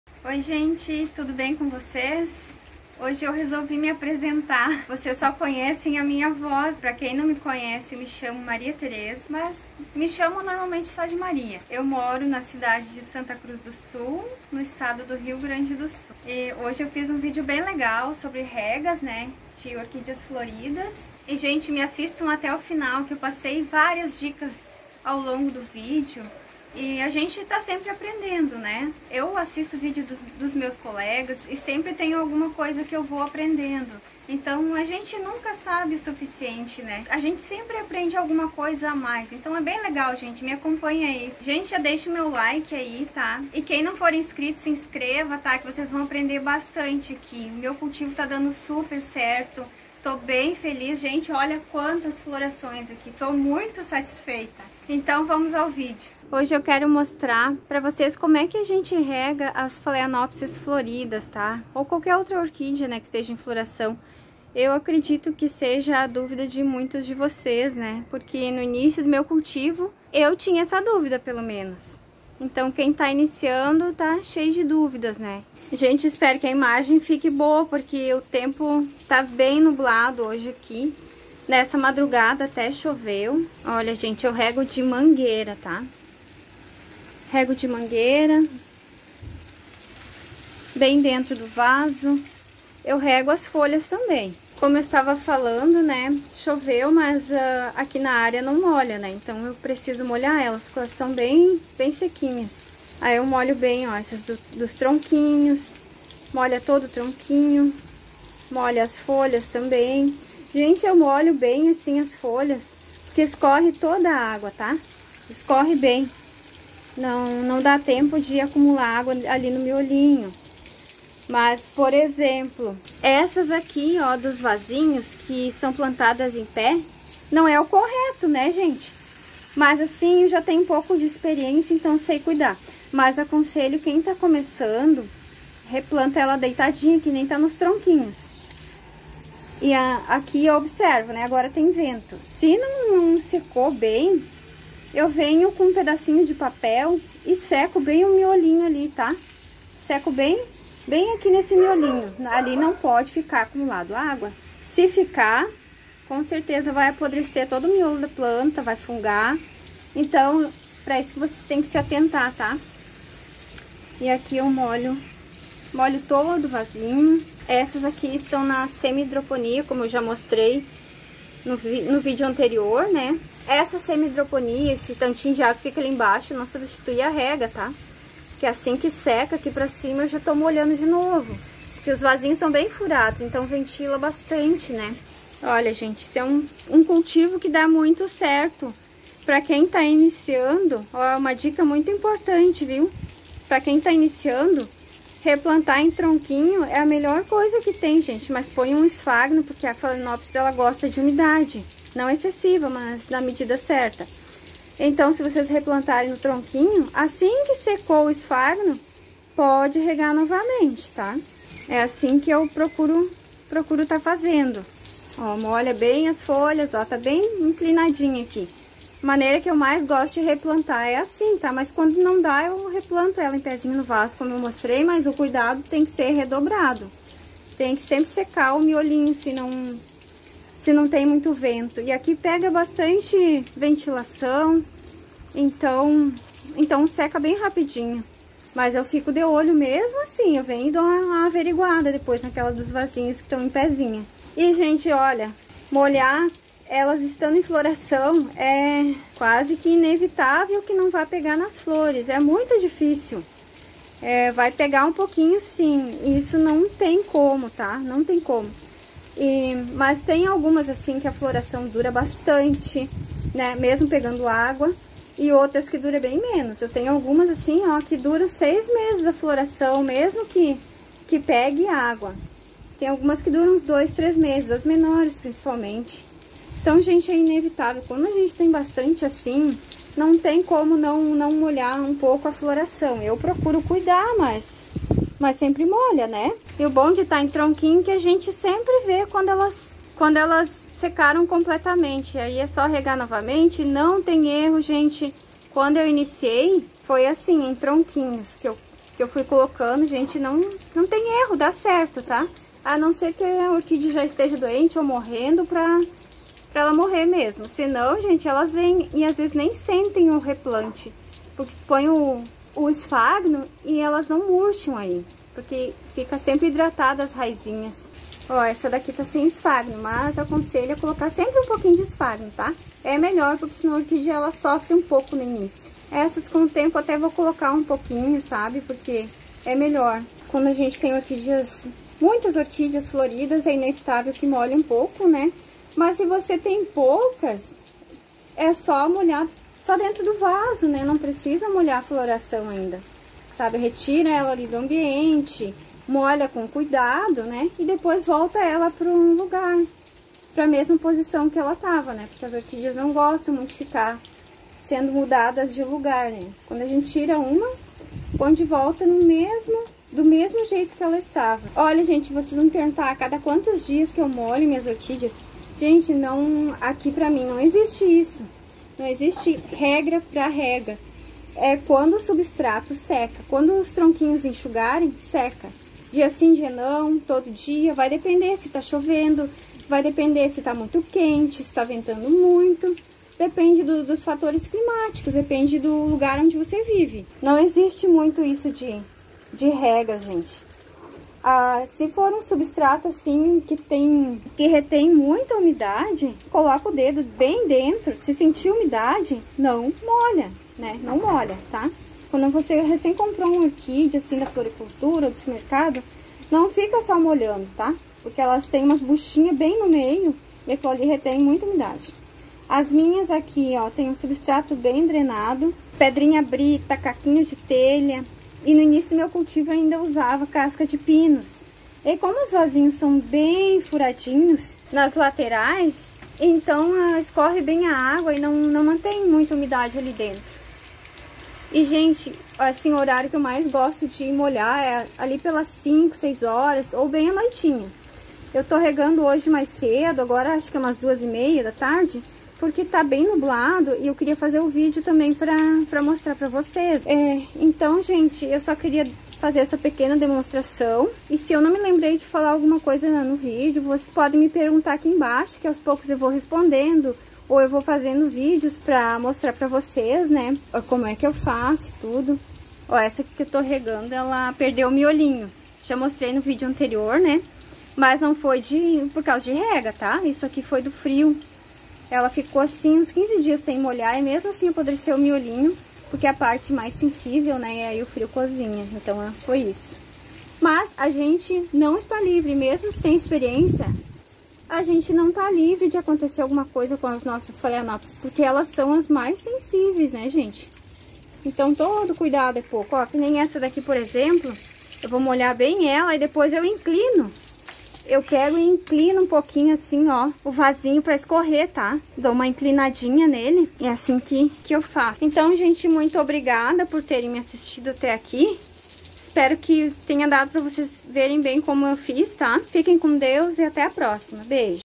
Como REGAR as Orquídeas Phalaenopsis FLORIDAS
No vídeo de hoje vou como regar as orquídeas phalaenopsis floridas. Inclusive passei diversas dicas para os iniciantes no cultivo de orquídea phalaenopsis.

Muitas pessoas tem dúvidas se pode molhar as folhas ou as flores das orquídeas quando as mesmas estão em floração. Até mesmo muitas dúvidas sobre regar as orquídeas estando plantadas em pé no vaso.

O melhor é sempre replantar a orquídea phalaenopsis inclinada no vaso para que a água escorra com facilidade e não se acumule no miolo, evitando que a orquídea apodreça ou desenvolva fungos.

Então se você quer saber como se deve regar as orquídeas durante a floração, e de quantos em quantos dias se deve regar as phalaenopsis, assista esse vídeo que expliquei bastante sobre assunto.

📲 Me siga também no Instagram: @mariaesuasorquideas

▶️ PLAYLISTS DE VÍDEOS DO CANAL QUE RECOMENDO ASSISTIR!